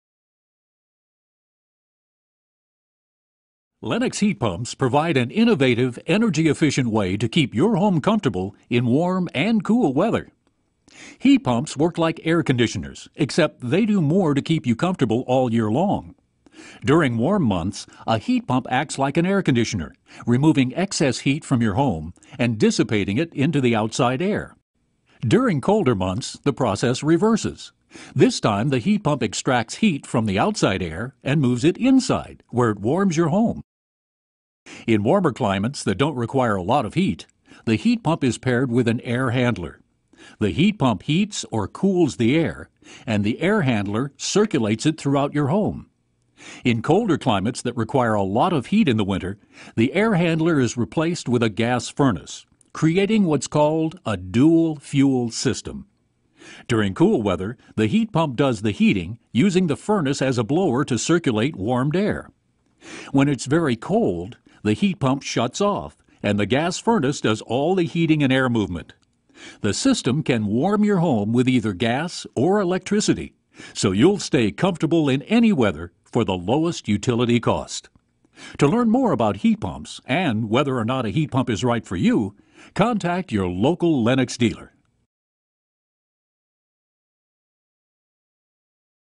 Lennox Heat Pump Technology Explained
Lennox heat pump. Explore how a Lennox heat pump works year-round to keep your home comfortable. This Lennox heat pump system includes a dual fuel system, which you can learn about in this video.

Intelligent Design Air Conditioning and Heating
2200 W. La Osa Street
Tucson, Arizona 85705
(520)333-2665